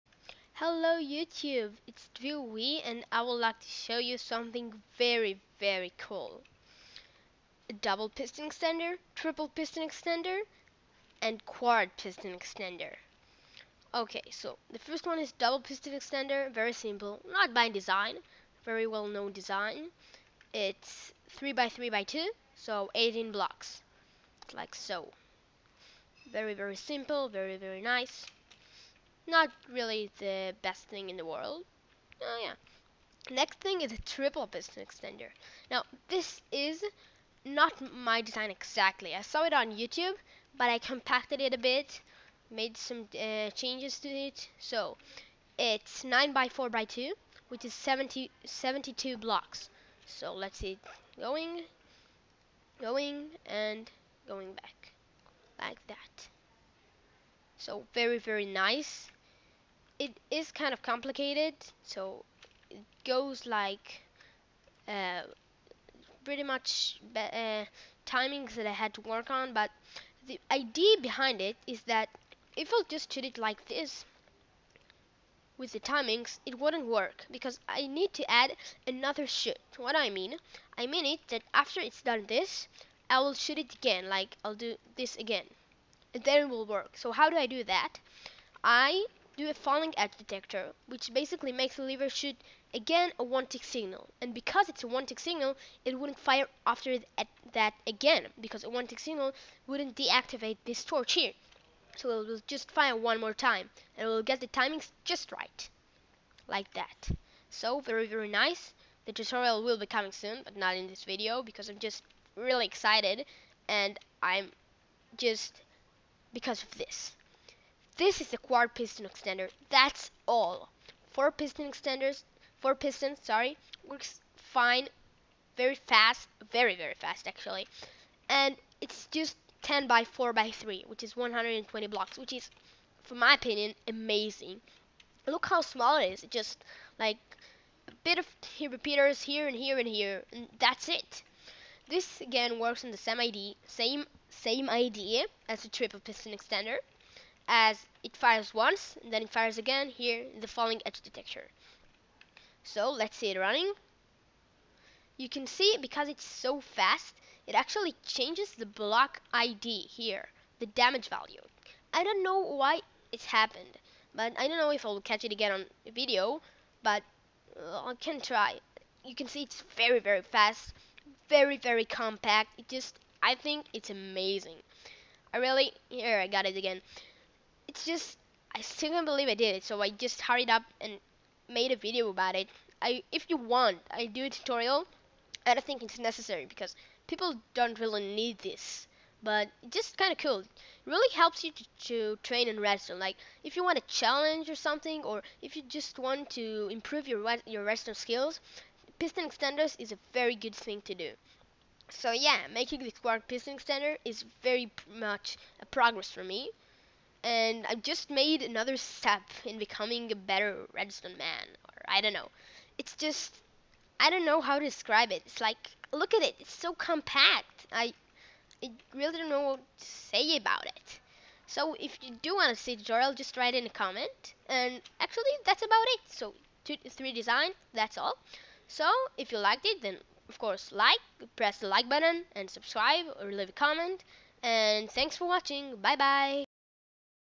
Double, Triple, Quadruple Piston extendrers!
The triple piston extender is not really mine, I saw it here and I compacted it:
And the quadruple pistone extender deisgn is completly mine =D
Whould you like to see a tutorial? write in the comments below!

I created this video and I own all the rights for it.